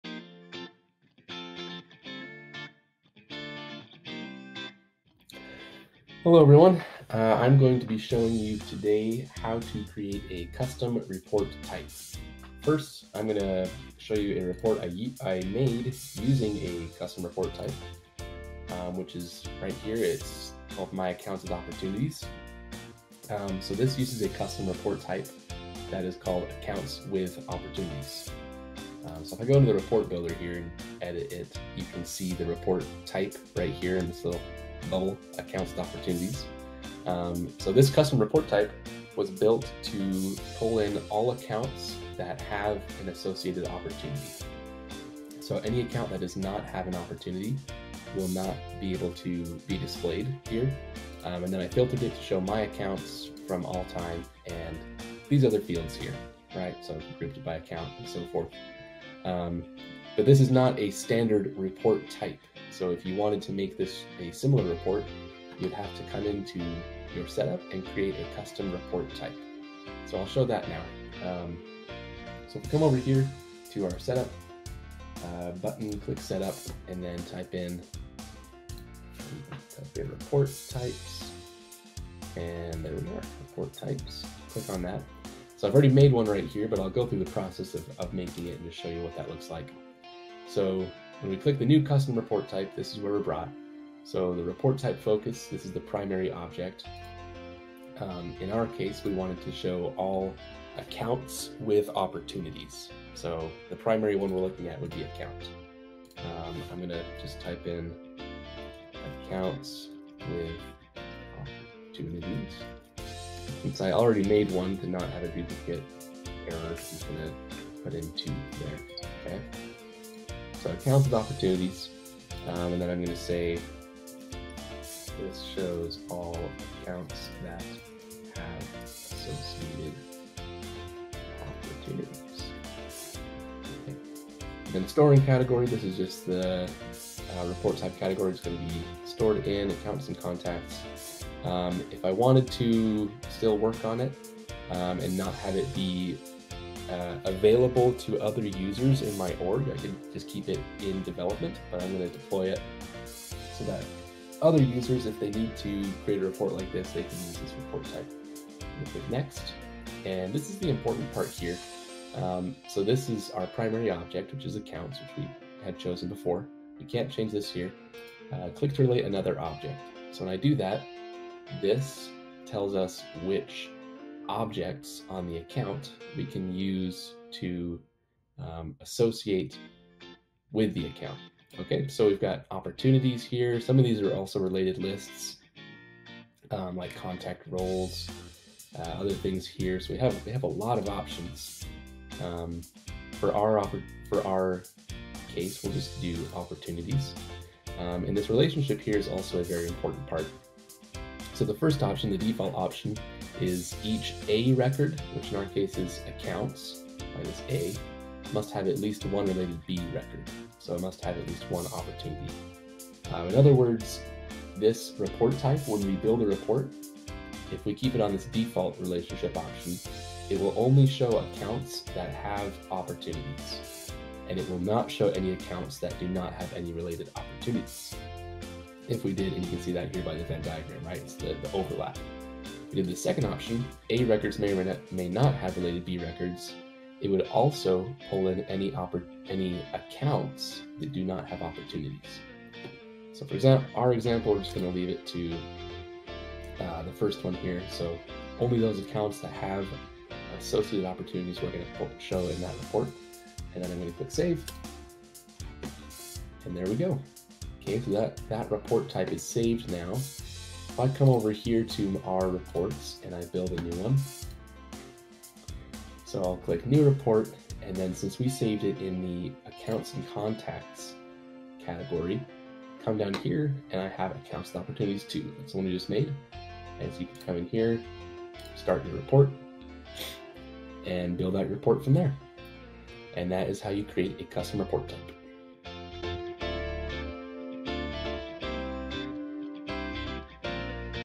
Report Types in Salesforce
Join Ben S are they show us all about custom report types and how to build them on salesforce